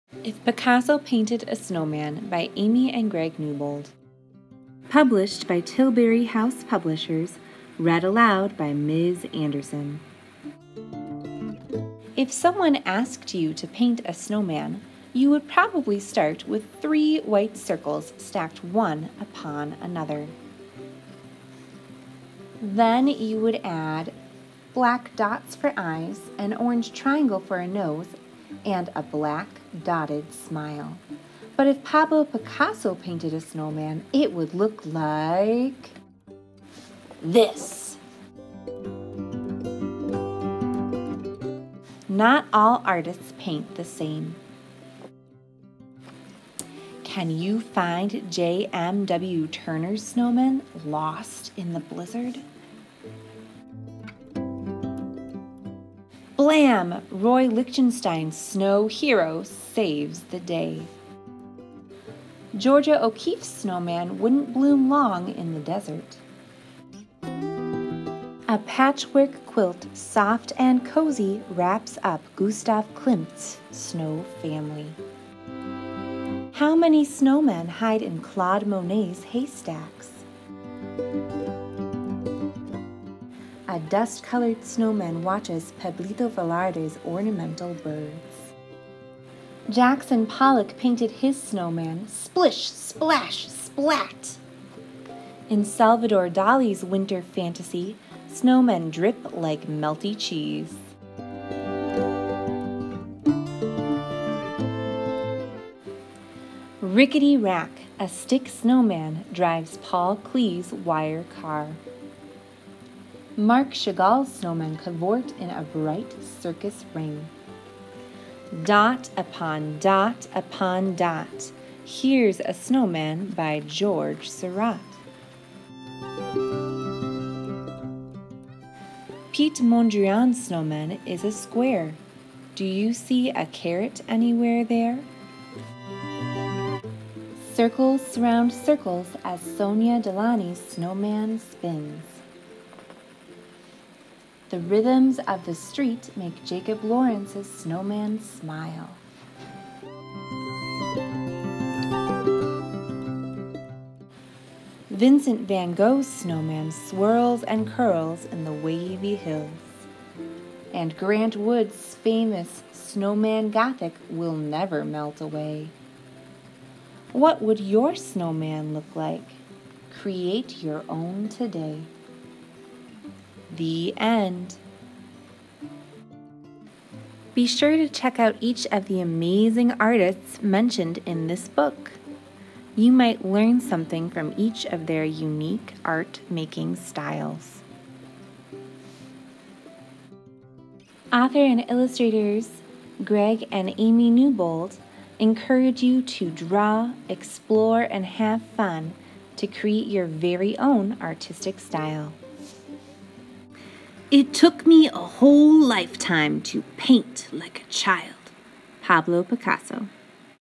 If Picasso Painted a Snowman Read Aloud
If Picasso Painted a Snowman, written and illustrated by Amy and Greg Newbold, Published by Tilbury House Publishers, Read by Ms. Anderson. This read aloud is created for school use and is intended for students.